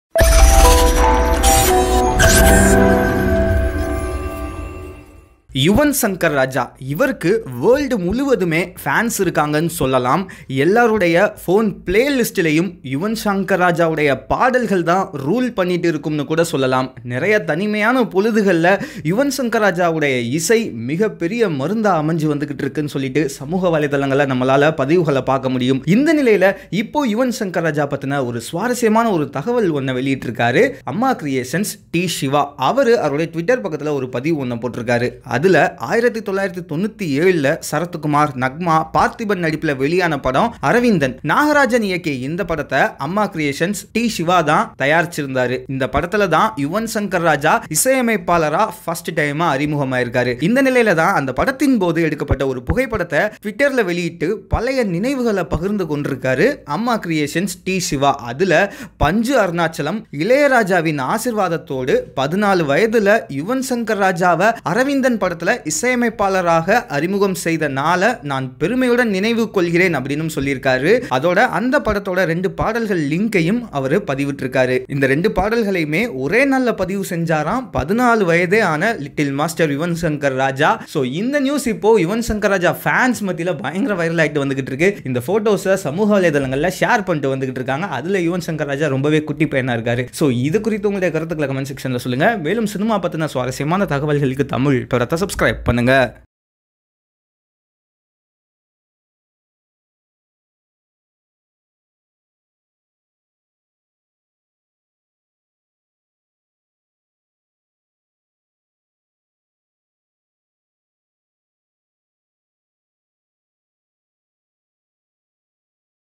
யுவன் ஷங்கர் ராஜா இசை அமைத்த முதல் திரைப்படம் | Amma creations | Siva | Ilayaraja | Thamizh Padam
யுவன் ஷங்கர் ராஜா இசை அமைத்த முதல் திரைப்படம். Watch this video on Thamizh Padam.தன் பதிநான்காம்  வயதில்  இசையின் மேல் மிகுந்த ஆற்றலும் அறிவும் கொண்டிருந்தார். அம்மா கிரேஷன்ஸ் சிவா அவர்கள் தனது ட்விட்டரில் பழைய நினைவுகளை நினைவுக்கூறும் வகையில் அரவிந்தன் இசையிட்டு விலையில் எடுத்த புகைபடத்தை பகிர்ந்துந்துள்ளார்!